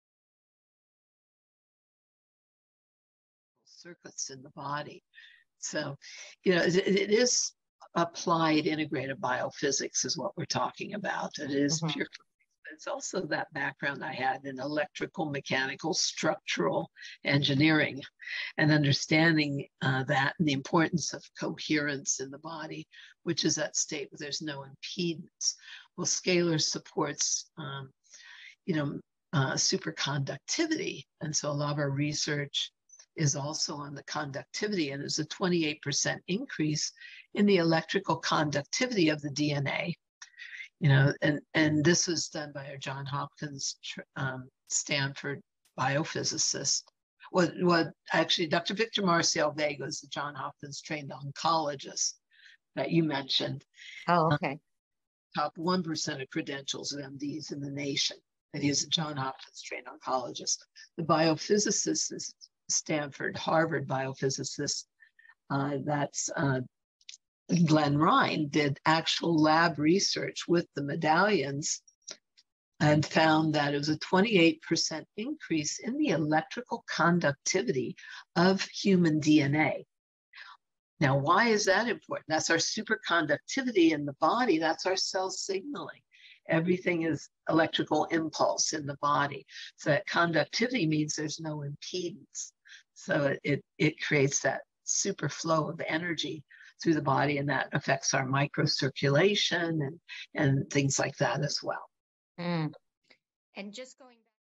Dr. Sandra Rose Michael describes the Medallions
Dr. Sandra Rose Michael, inventor of the EE System gives a short overview of the Medallions.

You can ask our staff while at NRG Plus in Huntley, IL about our selection ; )